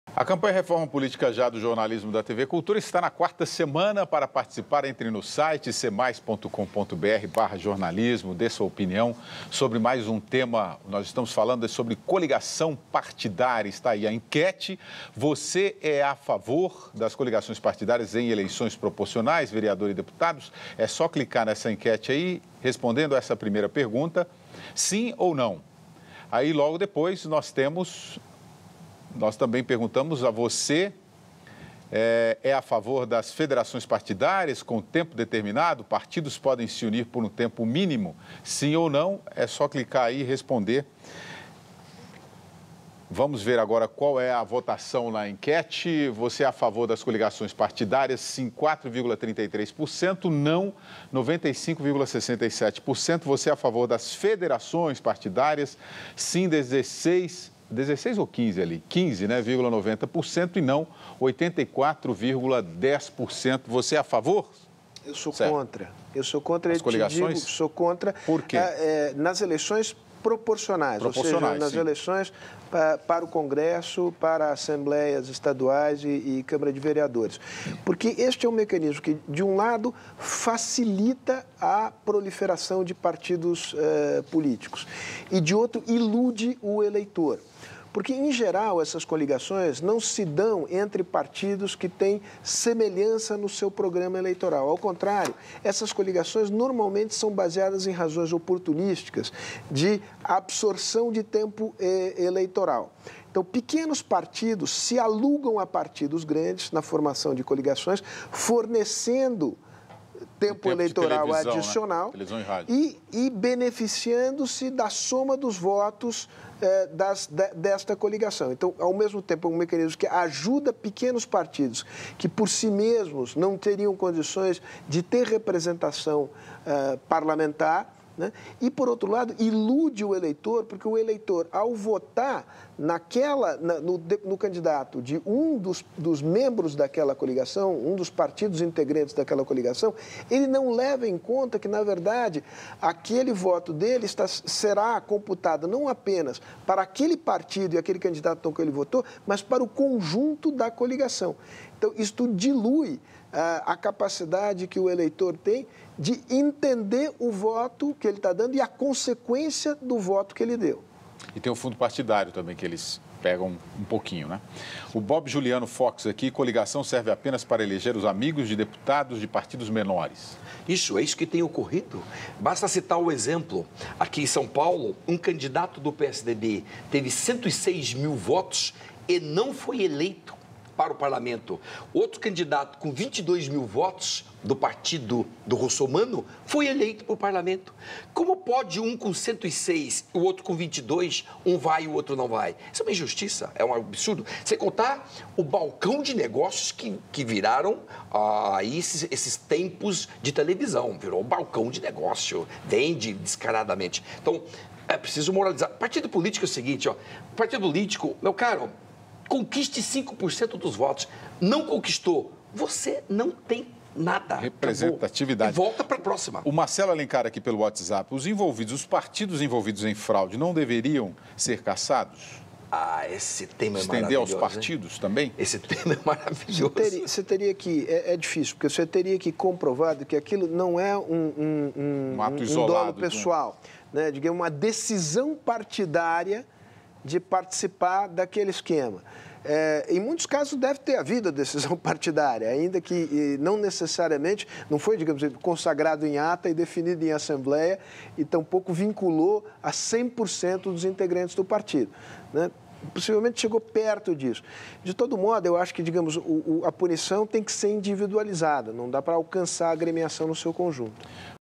Sérgio Fausto e Luiz Flávio Gomes debatem sobre Coligação Partidária - 01/12/2014
Sérgio Fausto e Luiz Flávio Gomes debatem sobre Coligação Partidária e Reforma Política